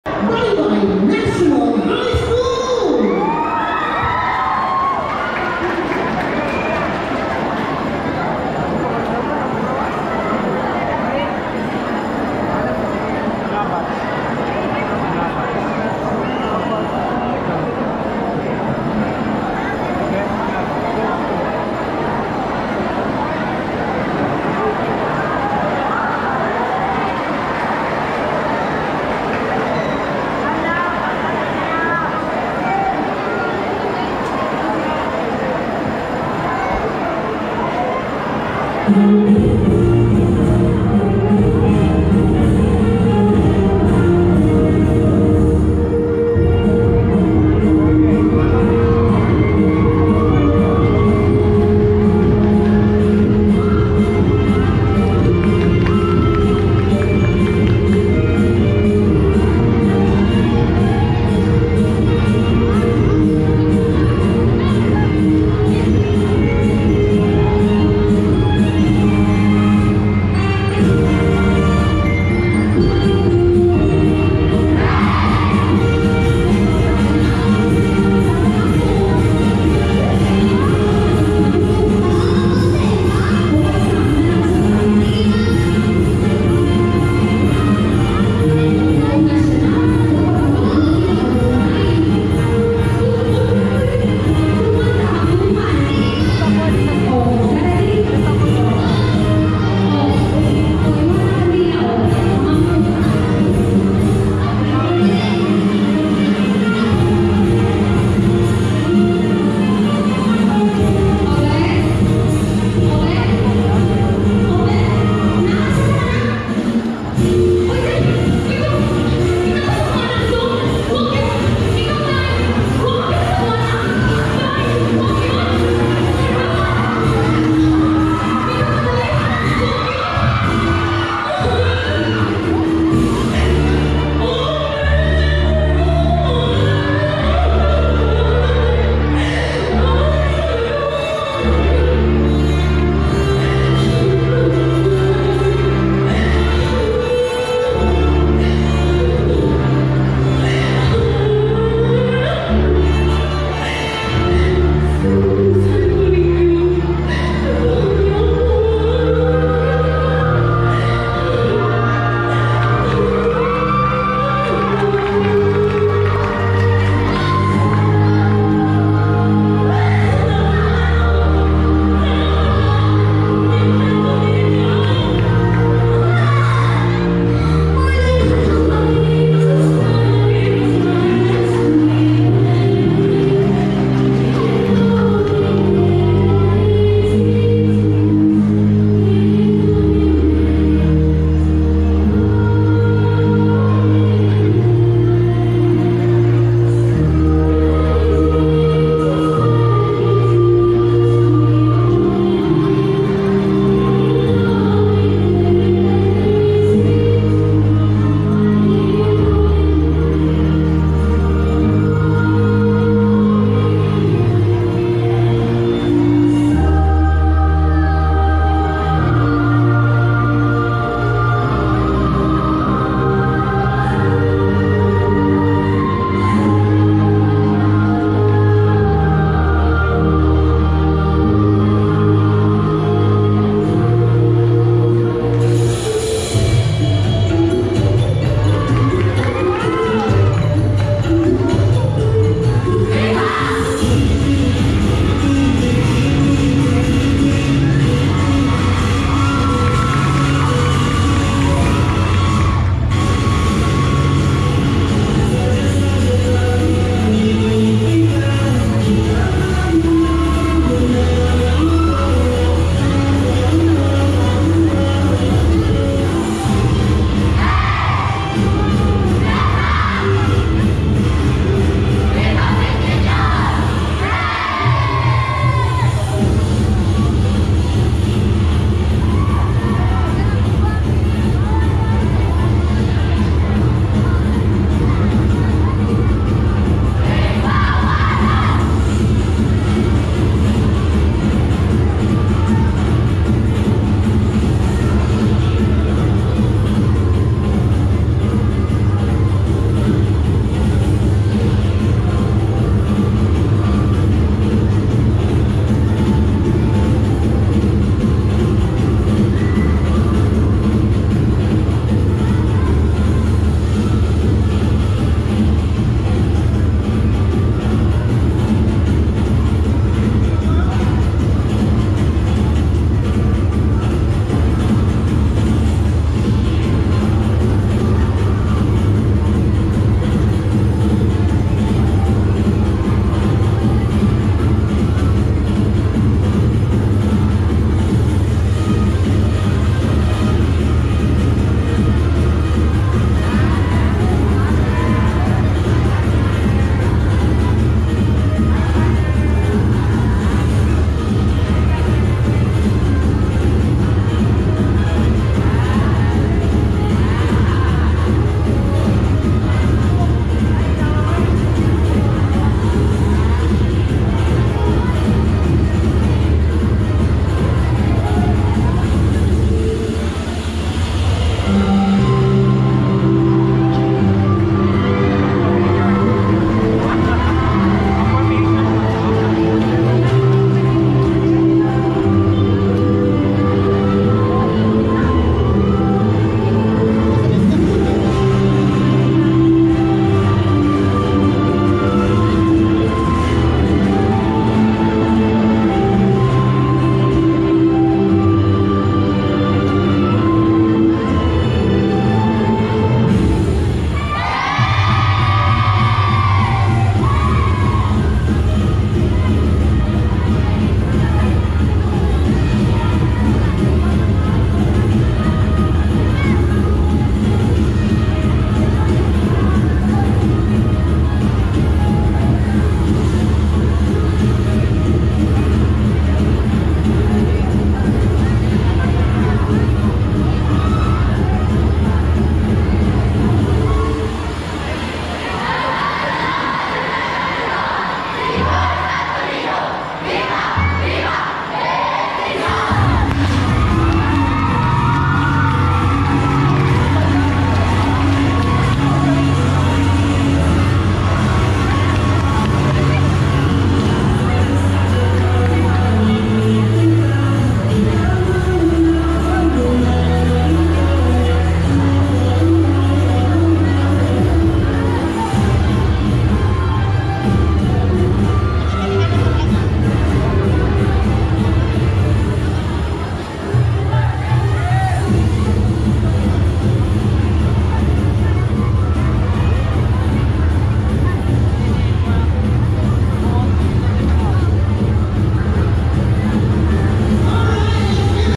"Grand Champion"🏆Baybay National High School Performance | Tigbawanon Festival 2023
Baybay National High School Performance 2023.Tigbawanon Festival is a traditional way to celebrate the fiesta of Patron Santo NIÑO. Our barangay Plaridel, Baybay City, Leyte is celebrating the fiesta of our patron Senior Santo. NIÑO last Saturday, January 21, 2023.
Happy Fiesta Plaridel, Baybay City, Leyte.
Viva Senior Sto. Niño! Viva Pit Senior! Viva! Viva! Pit Senior!!!🎈🎉✨

God Bless and Stay Safe Everyone. Thank You. 😘😘😘